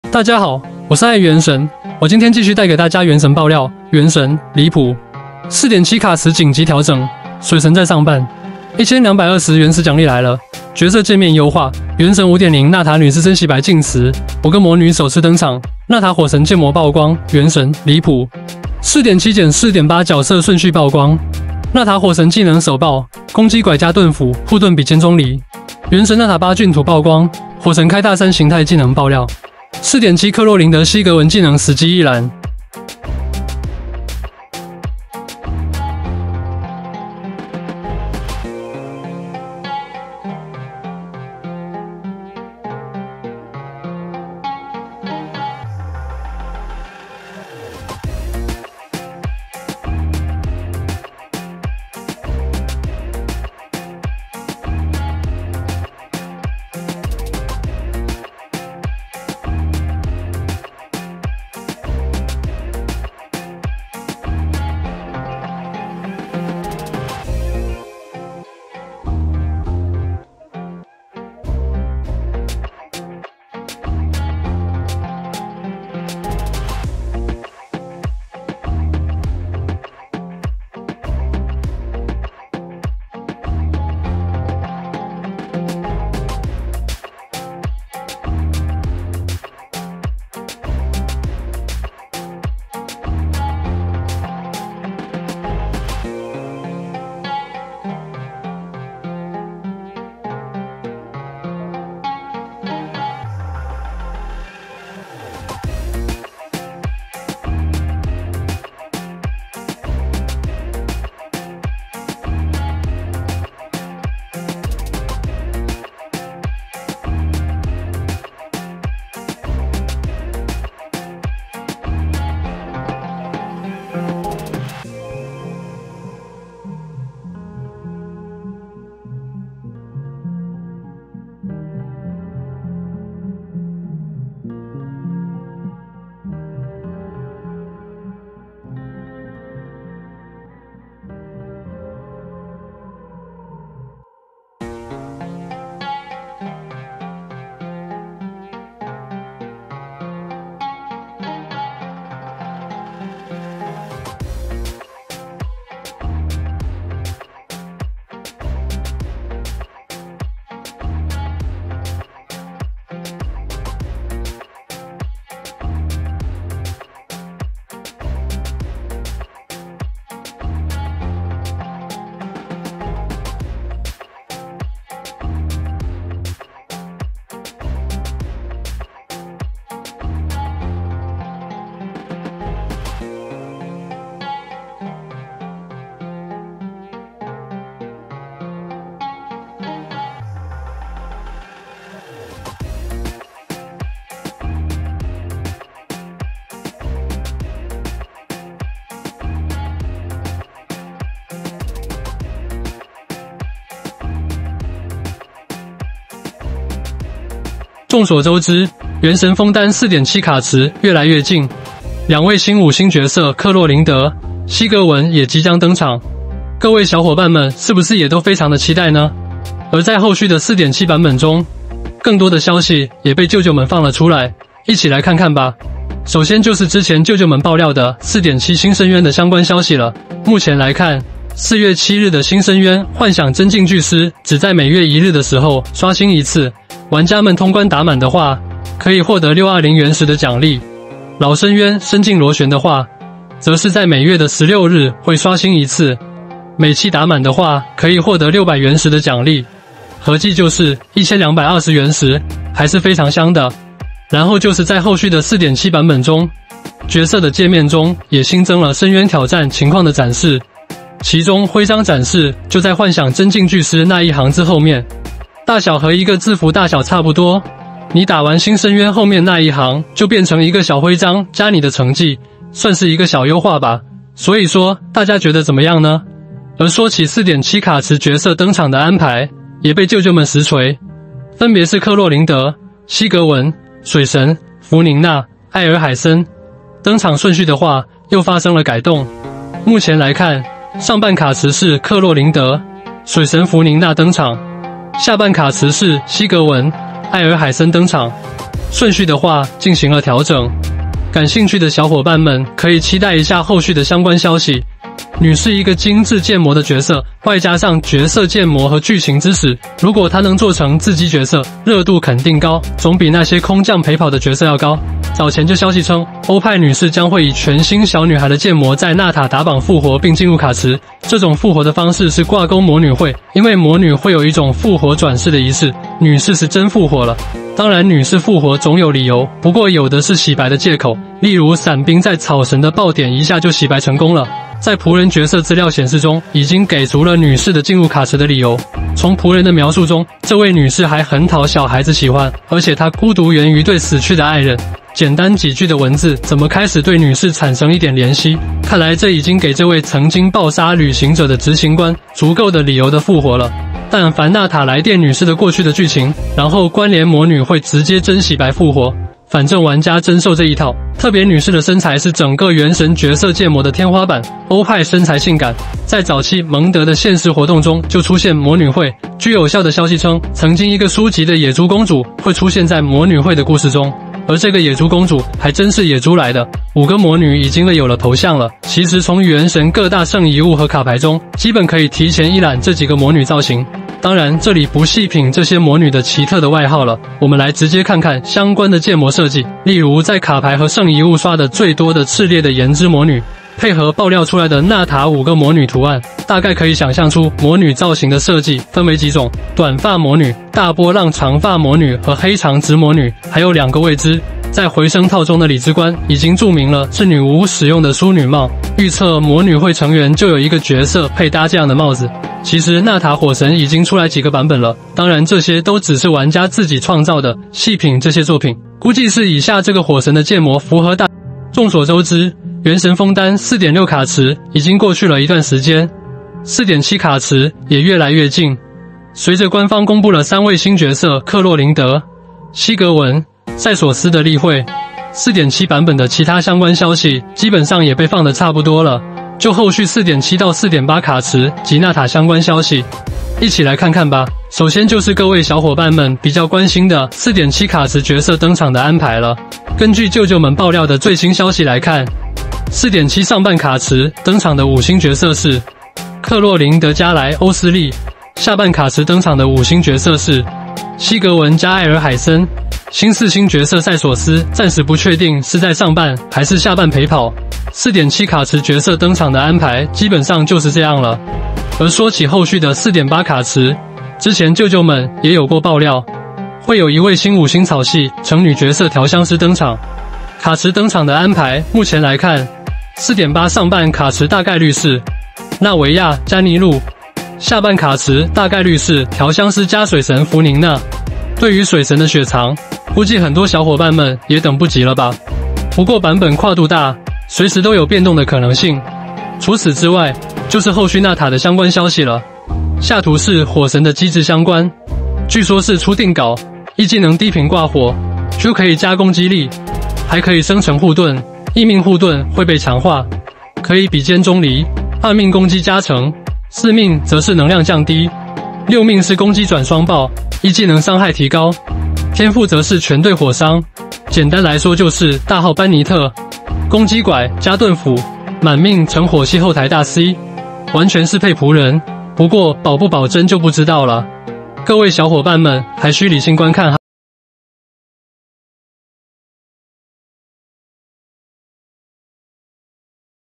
原神：離譜！ 4.7卡池緊急調整，水神在上半！ 1220原石獎勵來了，角色界面優化，原神5.0納塔女士真洗白進池，五個魔女首次登場，納塔火神建模曝光.
Genshin Impact: Outrageous! 4.7 The card pool has been adjusted urgently, and the water god is in the first half! The 1220 rough stone reward is here, the character interface is optimized, Genshin Impact 5.0 Ms. Nata is really clean and enters the pool, five witches make their debut, Nata Vulcan modeling is exposed, Genshin Impact: Outrageous! 4.7-4.8 characters revealed in sequence! Nata Vulcan's skills were revealed for the first time, attack and shield assist, and his shield is as good as Zhongli's? The picture of the eight horses of the original god Nata was revealed, and it was revealed that the Vulcan's skills in the third form were revealed! 4.7 Actual list of Claudine Higwen’s skills!